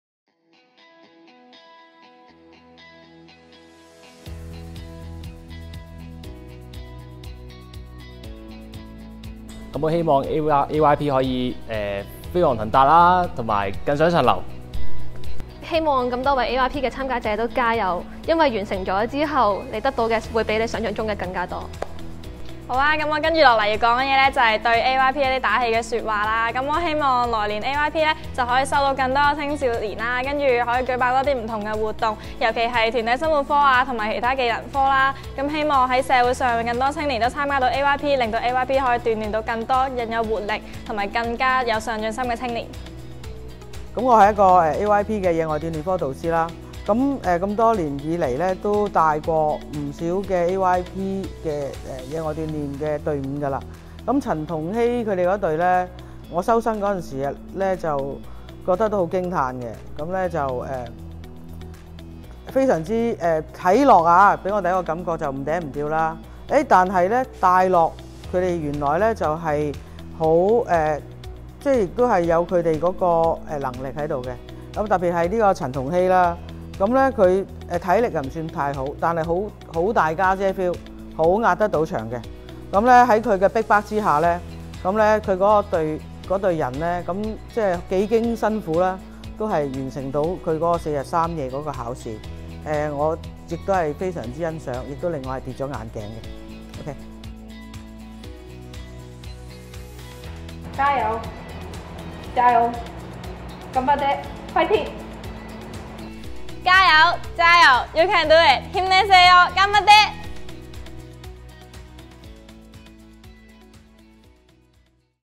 【金章頒獎典禮2017 得獎者及嘉賓訪問- 打氣篇】
金章是獎勵計劃的最高榮譽，鍛鍊過程殊不簡單。所有AYP金章得獎者須最少投放12個月的時間克服五科的考驗和挑戰，包括最少100小時的社會服務、12個月的技能訓練、4日3夜步行60公里的野外鍛鍊評核旅程、最少持續18個星期的康樂體育活動，及最少連續5日4夜的外宿團體活動。
成功完成金章級的青年能夠自發並持續參與五科活動，懂得善用餘暇，實在難能可貴。讓我們一起看看他們不平凡的體驗，細味他們的感受，領略箇中點滴！

The Gold Award is the highest level of the Award Scheme, All Gold Award recipients have to fulfill various requirements for five sections of activities.
Let's enjoy the sharing told by our Gold Award recipients!